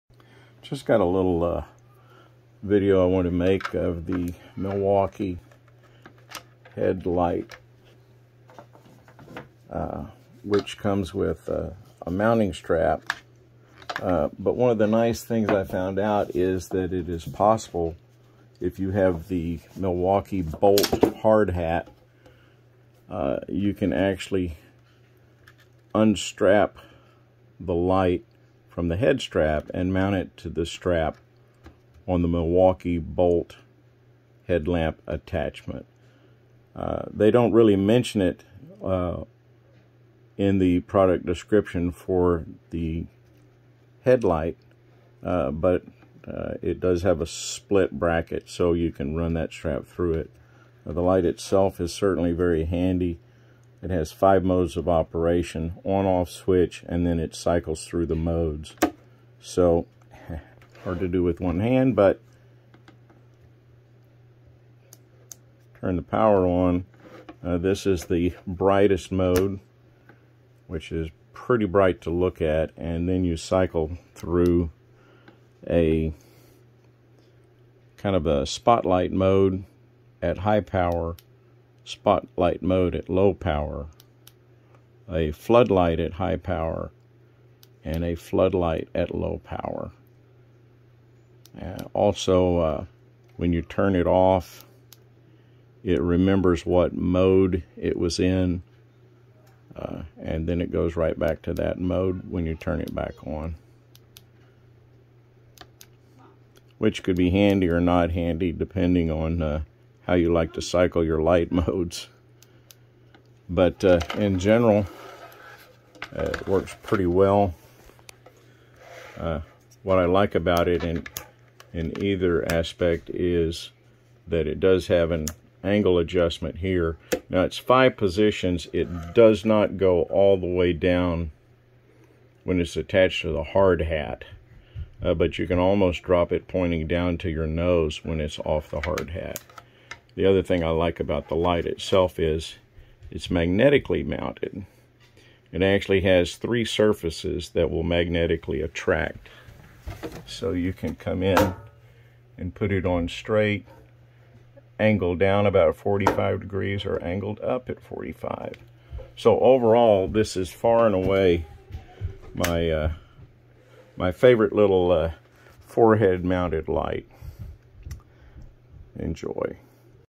Milwaukee 2012R, 450 Lumens Internal Rechargeable Magnetic Headlamp and Task Light, Red
SUMMARY: The nicest headband light I’ve owned to date.
BACKGROUND: I have several headband lights, some cheap, some from big-name companies; but the Milwaukee 2012R is the nicest by far.
FIT AND FINISH: The quality of materials is comparable to my other Milwaukee tools and gear and is totally acceptable for a light in this price point. The head is very compact but easy to operate and the quick-detach magnetic mount capability is very handy.
FEATURES: For a small light there is a lot to say.
• 450 Lumens of TRUEVIEW High-Definition Output
o (From an online source) Milwaukee says that their TRUEVIEW LED products have a CRI of 85 (the maximum CIR is 100.) That is not what would be considered a “very high CRI,” but it is in the top 15 percent. Milwaukee’s TRUEVIEW LED lighting products have a color temperature of 4000K, which is not quite what would be considered daylight (~5600K). 4000K is between warm and cool ends of the white light spectrum and is quite pleasing.
• Spot, Flood and Hybrid Beam Options for the Ideal Intensity and Coverage
o Two LED lights with the ability to switch either or both on at the same time.
• 5 Light Output Modes:
o Hybrid Spot/Flood mode: 450-Lumens- 2.5 hours
o Spot High: 250-Lumens- 4 hours
o Spot Low: 50-Lumens- 8 hours
o Flood High mode: 250-Lumens- 4 hours
o Low: 50-Lumens- 14 hours
• Magnetic light head detaches from the strap holster
o Pop the light off for hand-held or magnetic mounting (see photos)
• Can magnetically mount the light head in 3 positions (see photos)
• Rechargeable via supplied 2-foot Micro-USB cable
o I’m converting all my mobile lighting to rechargeable; it beats grabbing a light that has dead (and leaking) batteries.
• Monitor runtime with an on-board battery indicator (Green, Yellow, Red)
• IP54 rated as weather and dust resistant and survives impacts from up to 2 meters
• 2-year warranty
• 5-Position Adjustable Light Head strap holster for Directing Light
o The holster uses a detent type angle adjustment which is handy when you can’t get your head positioned at the right angle.
• Soft, sweat absorbing microfiber padding on the forehead of the adjustable elastic head band is very comfortable.
• Light Holster can be removed from the head strap and attached to the light bracket on Milwaukee BOLT hardhats.
o It’s a bit of a pain to do, but if you were going to use the light almost always on a hardhat this is a handy feature (see photos.) Because the light mounts to the holster magnetically you can still remove it from the hardhat to mount somewhere else.
• 2-year Warranty
• Includes: (1) 2012 Rechargeable Magnetic Headlamp and Task Light w/ (1) 2-foot heavy duty cable and (4) Universal hard hat clips
o If you don’t have a Milwaukee BOLT hardhat with a light bracket on the front you can use the clips to put the head strap on your hardhat. (I have already misplaced the clips.)

OPERATION: Two buttons: on/off, and mode. The mode button cycles through the 5 lighting modes described above. The light does remember the last lighting mode used and will return to that mode when the light is turned on again. The light WILL NOT TURN ON while charging.

SUMMARY: Small, light, with good features. Perfect for my DIY work.